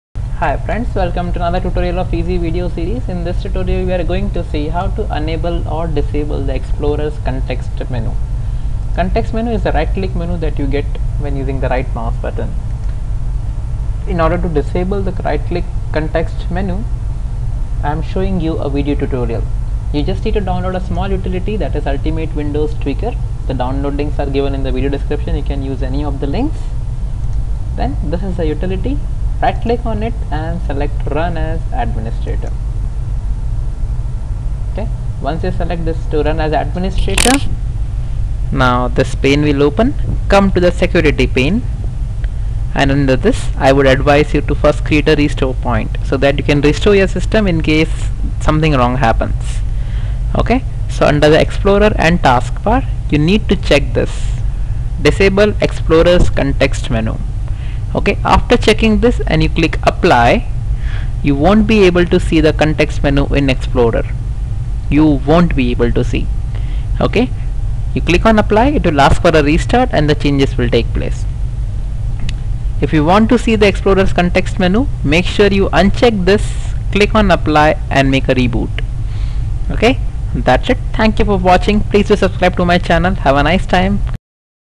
Enable OR Disable Explorer Context Menu Security Tweaks Windows 8 and 8 1 A Step by Step Tuto
Ultimate Windows Tweaker 3

Create Restore Point Before Any Tweak !